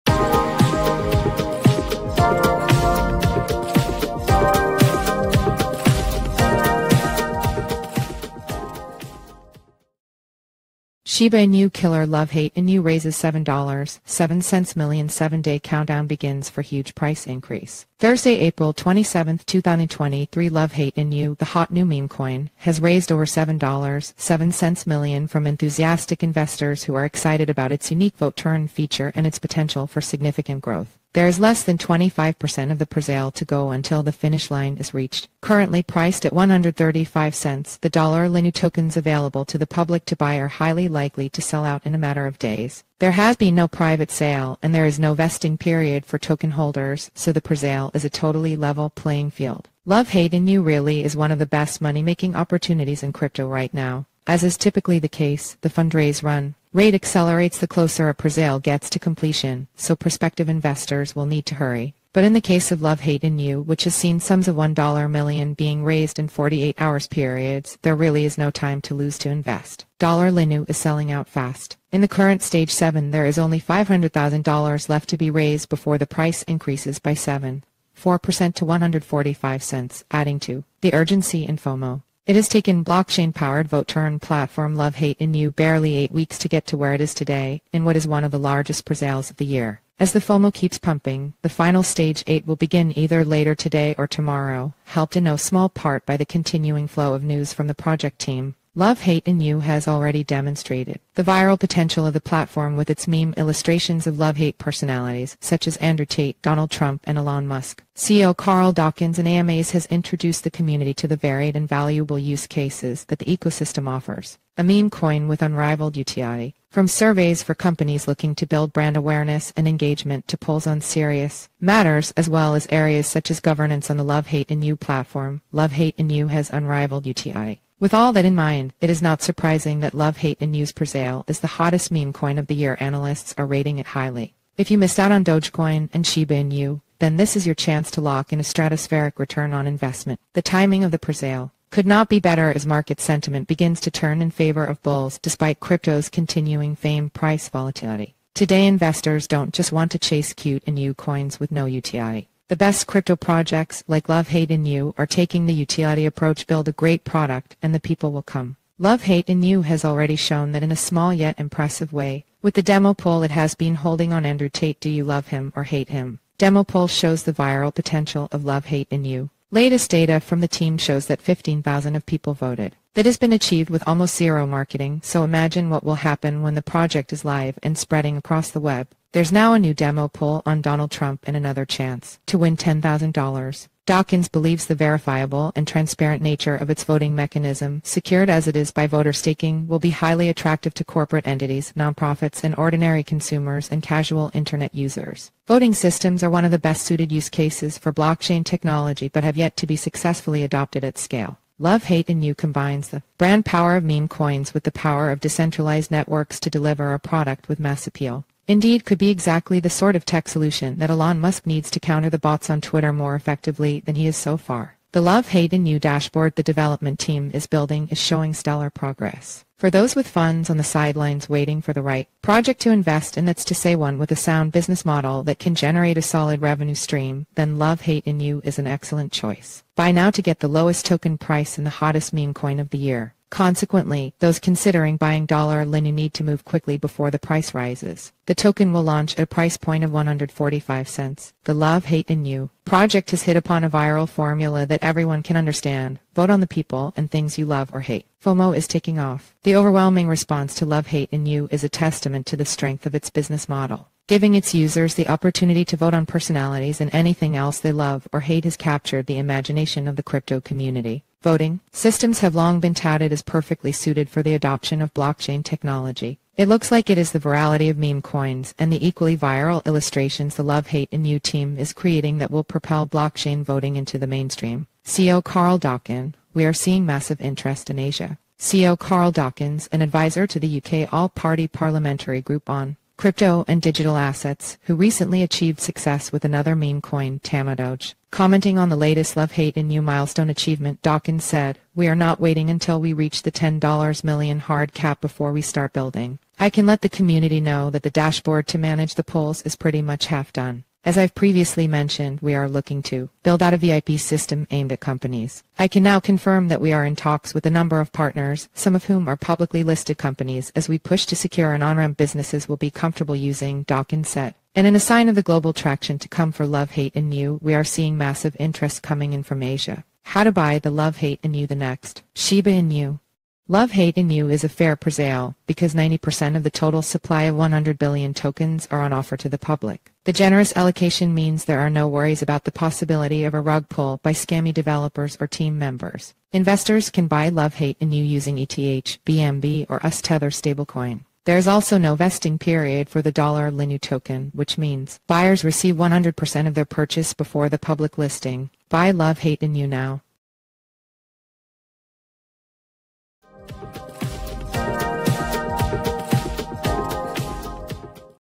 Shiba Inu Killer Love Hate Inu Raises $7.7 Million – 7 Day Countdown Begins for Huge Price Increase
Thursday April 27th 2023 – Love Hate Inu, the hot new meme coin, has raised over $7.7 million from enthusiastic investors who are excited about its unique vote-to-earn feature and its potential for significant growth.

There is less than 25% of the presale to go until the finish line is reached. Currently priced at $0.000135, the $LHINU tokens available to the public to buy are highly likely to sell out in a matter of days.

There has been no private sale and there is no vesting period for token holders, so the presale is a totally level playing field.

Love Hate Inu really is one of the best money-making opportunities in crypto right now.

As is typically the case, the fundraise run rate accelerates the closer a presale gets to completion, so prospective investors will need to hurry.

But in the case of Love Hate Inu, which has seen sums of $1 million being raised in 48 hours periods, there really is no time to lose to invest.

$LHINU is selling out fast.

In the current Stage 7 there is only $500,000 left to be raised before the price increases by 7.4% to $0.000145, adding to the urgency and FOMO.

It has taken blockchain-powered vote-to-earn platform Love Hate Inu barely eight weeks to get to where it is today, in what is one of the largest presales of the year.

As the FOMO keeps pumping, the final Stage 8 will begin either later today or tomorrow, helped in no small part by the continuing flow of news from the project team.

Love Hate Inu has already demonstrated the viral potential of the platform with its meme illustrations of love-hate personalities, such as Andrew Tate, Donald Trump and Elon Musk.

CEO Carl Dawkins in AMAs has introduced the community to the varied and valuable use cases that the ecosystem offers.

A meme coin with unrivaled utility.

From surveys for companies looking to build brand awareness and engagement, to polls on serious matters, as well as areas such as governance on the Love Hate Inu platform, Love Hate Inu has unrivaled utility.

With all that in mind, it is not surprising that Love Hate Inu’s presale is the hottest meme coin of the year – analysts are rating it highly.

If you missed out on Dogecoin and Shiba Inu, then this is your chance to lock in a stratospheric return on investment.

The timing of the presale could not be better as market sentiment begins to turn in favor of bulls, despite crypto’s continuing famed price volatility.

Today investors don’t just want to chase cute Inu coins with no utility.

The best crypto projects, like Love Hate Inu, are taking the utility approach – build a great product and the people will come.

Love Hate Inu has already shown that in a small yet impressive way with the demo poll it has been holding on Andrew Tate – do you love him or hate him?

Demo poll shows the viral potential of Love Hate Inu.

Latest data from the team shows that 15,000 of people voted.

That has been achieved with almost zero marketing, so imagine what will happen when the project is live and spreading across the web.

There’s now a new demo poll on Donald Trump and another chance to win $10,000.

Dawkins believes the verifiable and transparent nature of its voting mechanism, secured as it is by voter staking, will be highly attractive to corporate entities, nonprofits and ordinary consumers and casual internet users.

Voting systems are one of the best-suited use cases for...